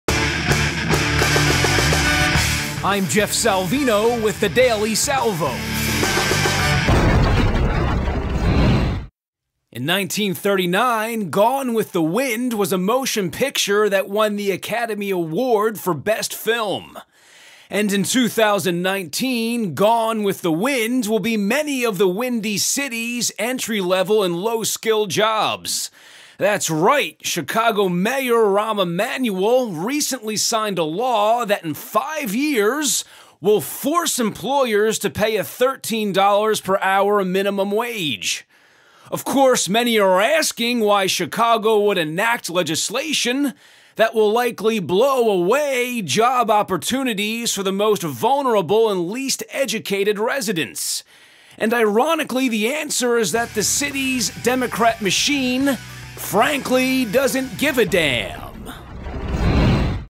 The Daily Salvo for December 11, 2014 - "Gone With the Wind"
Chicago Mayor Rahm Emanuel doesn’t seem to ‘give a damn’ that a $13/hour minimum wage will kill entry-level and low-skill jobs for the most vulnerable and least educated people.